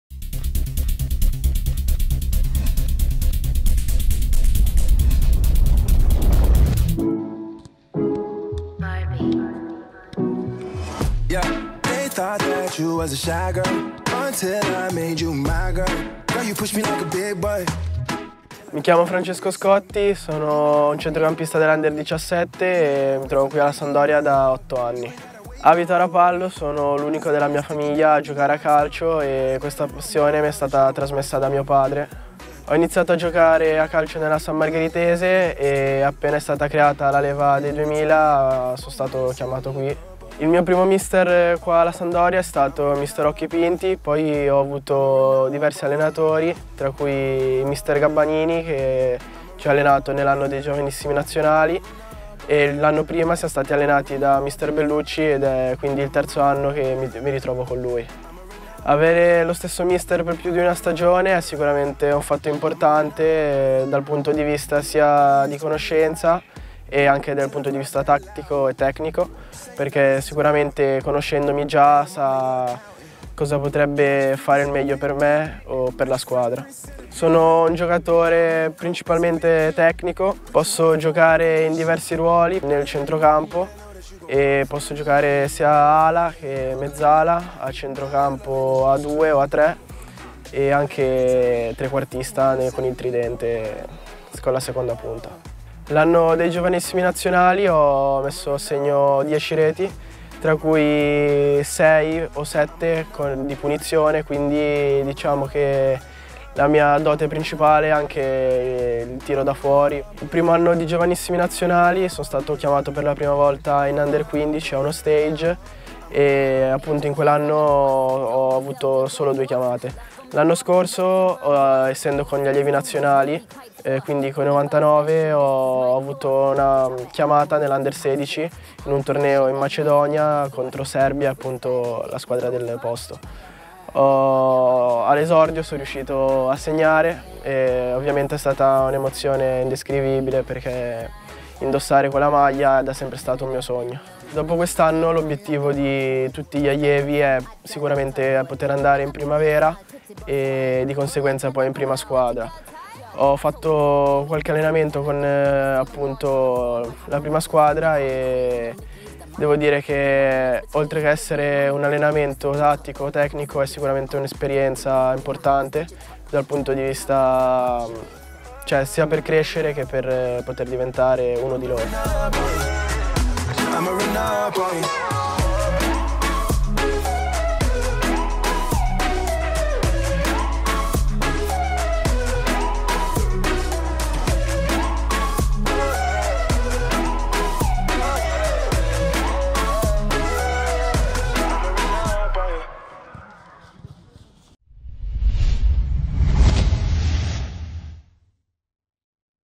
Academy: parla Scotti, il veterano dell'U17
Passano gli anni ma otto son lunghi. Francesco Scotti, classe 2000, è un veterano dell'Academy. Da otto stagioni fa parte del vivaio blucerchiato e quest'anno si allena alle dipendenze di Claudio Bellucci per la terza volta. «Avere lo stesso allenatore per più campionati è un vantaggio - spiega a Samp TV -. Mi considero un centrocampista duttile, dotato di tecnica e con un buon tiro da fuori, ma con il lavoro spero di migliorare fisico e continuità di rendimento».